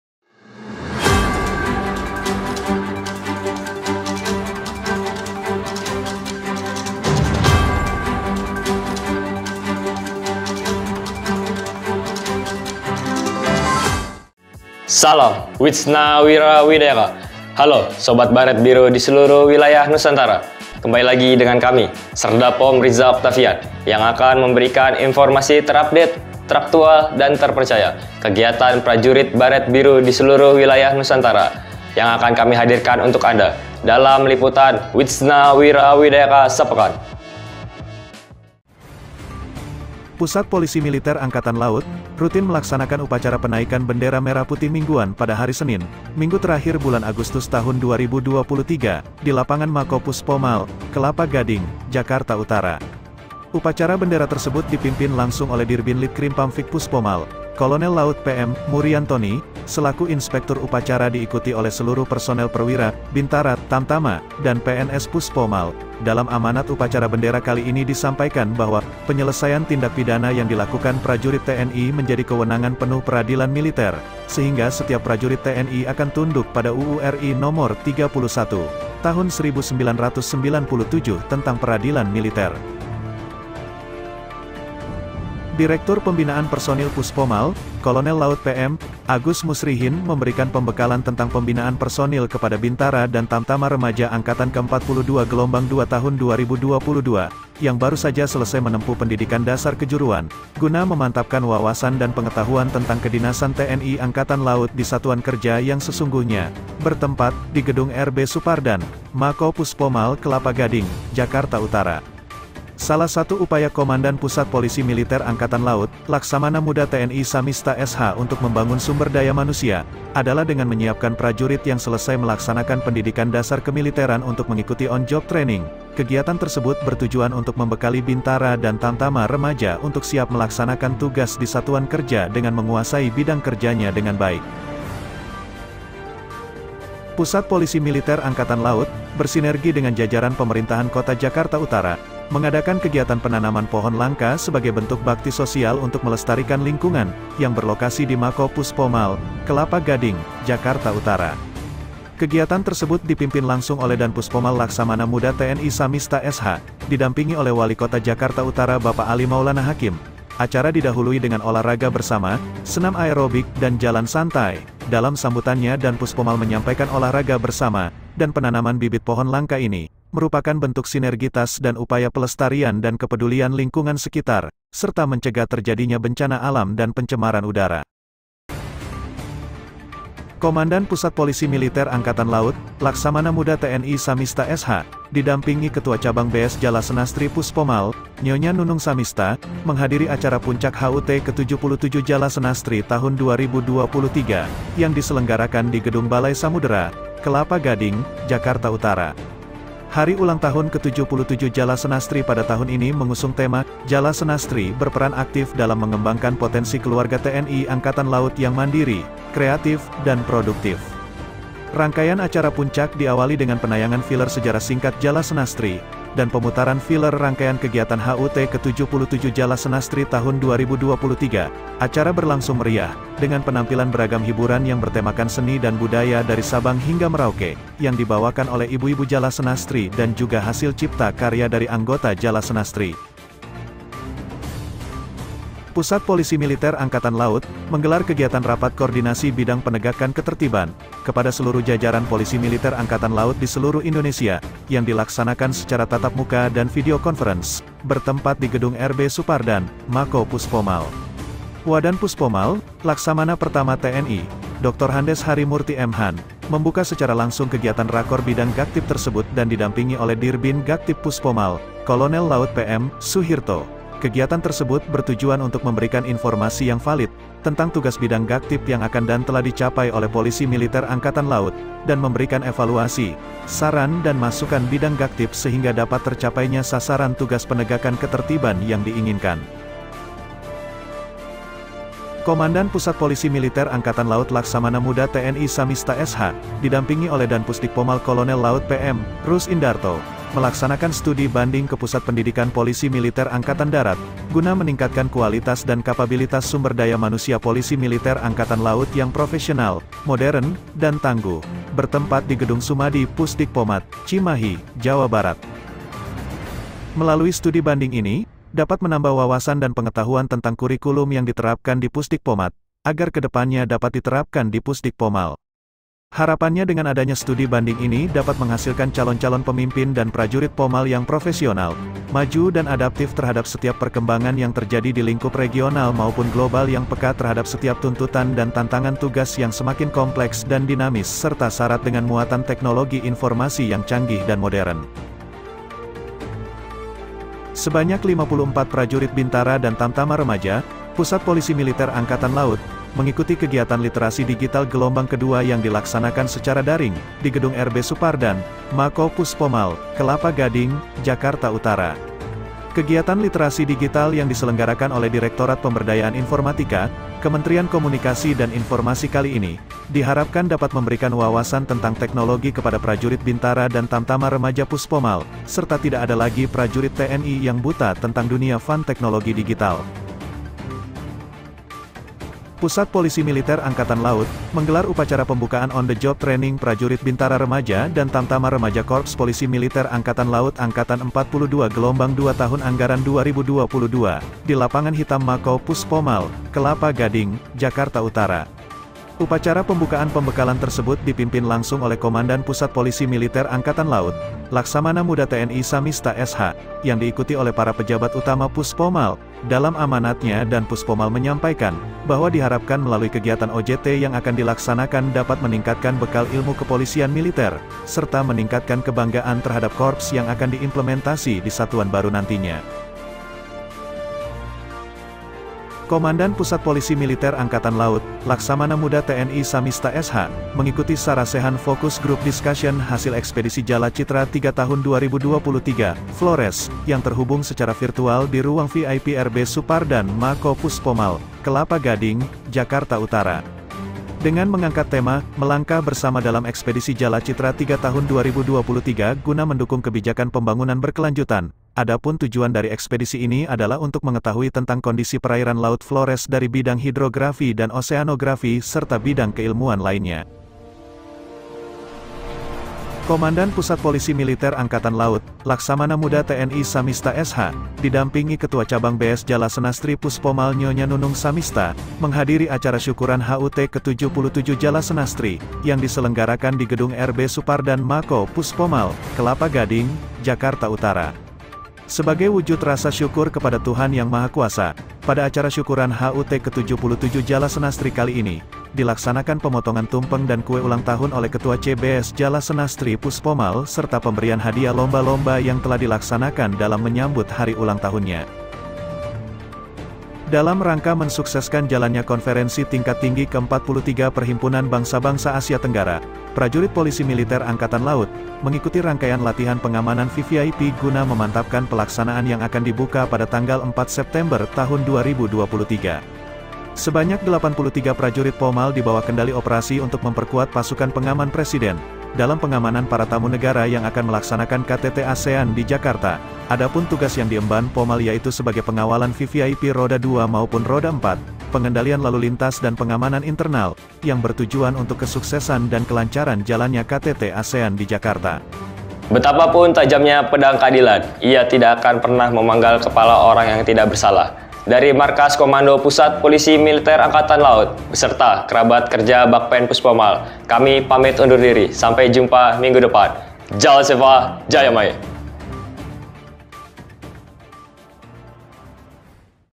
BERITA L3W BULAN SEPTEMBER MINGGU KE 1 TAHUN 2023
Berita Mingguan L3W edisi  bulan September Minggu ke-1 Tahun 2023, dengan berita

1. SETIAP PRAJURIT TNI TUNDUK PADA PERADILAN MILITER ”, AMANAT UPACARA BENDERA MINGGUAN DI MAKO PUSPOMAL

2. PAHAMI BIDANG PENUGASAN, DIRBINPERS PUSPOMAL BERIKAN PEMBEKALAN TENTANG PEMBINAAN PERSONAL

3. PUSPOMAL DAN PEMKOT JAKARTA UTARA BERSINERGI MELAKSANAKAN PENGHIJAUAN PENANAMAN POHON LANGKA

4. PUNCAK PERAYAAN ACARA HARI ULANG TAHUN JALASENASTRI KE-77 TAHUN 2023

5. SELARASKAN PERSEPSI DALAM BIDANG PENEGAKKAN KETERIBAN, PUSPOMAL GELAR RAPAT KOORDINASI

6. WUJUDKAN SDM POMAL YANG PROFESIONAL, DANPUSPOMAL STUDI BANDING KE PUSDIK POMAD

7. MENAMBAH WAWASAN TENTANG TEKNOLOGI, PRAJURIT BINTARA DAN TAMTAMA REMAJA PUSPOMAL MENGIKUTI LITERASI DIGITAL

8. KETAHUI KONDISI PERAIRAN FLORES DANPUSPOMAL MENGIKUTI SARASEHAN JALA CITRA 3 SECARA VICON

9. SIAPKAN PERFORMA TERBAIK PADA PELAKSANAAN PENGAMANAN DAN PENGAWALAN KTT ASEAN KE-43 TAHUN 2023

10. MANDIRI, KREATIF DAN PRODUKTIF, CBS JALASENASTRI PUSPOMAL LAKSANAKAN SYUKURAN HUT KE-77

11. SUKSESKAN KTT ASEAN KE-43, PRAJURIT POMAL MENGIKUTI LATIHAN PENGAMANAN VVIP